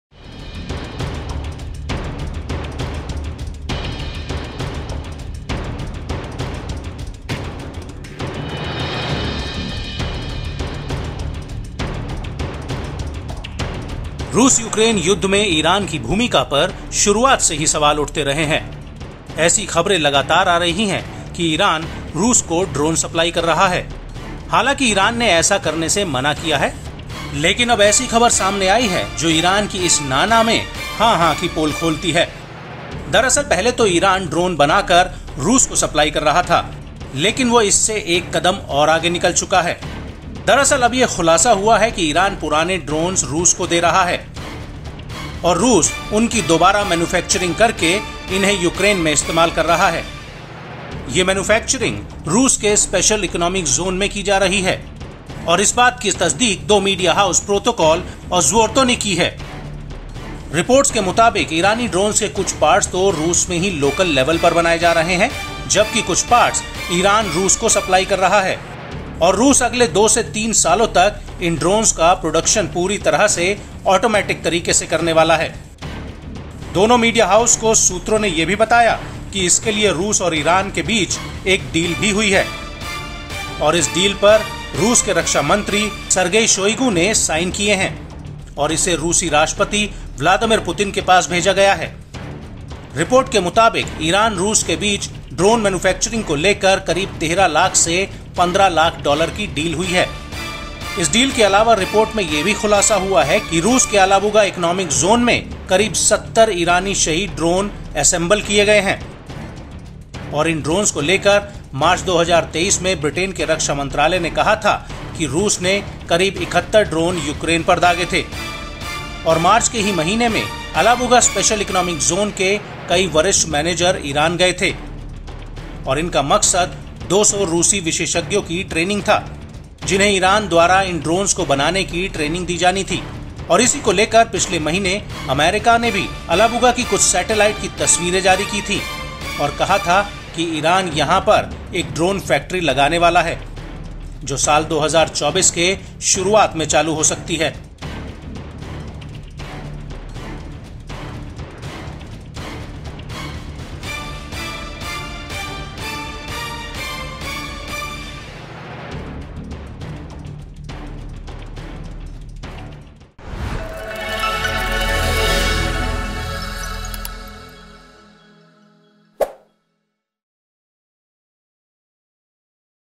Russia Ukraine War: Iranian Drone की खुल गई पोल | Russia Manufacturing | Latest News
Russia is doing Iranian Drone Manufacturing in its exclusive economic zone with Iran. These drones are being used in Ukraine War. According to media reports, a deal worth $1.2-1.5 million has been signed between the two countries for drone manufacturing.

Russia Iran के साथ मिलकर अपने विशेष आर्थिक क्षेत्र में Iranian Drone Manufacturing कर रहा है. इन Drone का इस्तेमाल Ukraine War में किया जा रहा है. मीडिया रिपोर्ट्स के मुताबिक, दोनों देशों के बीच ड्रोन मैन्युफैक्चरिंग को लेकर 12-15 लाख डॉलर की डील हुई है.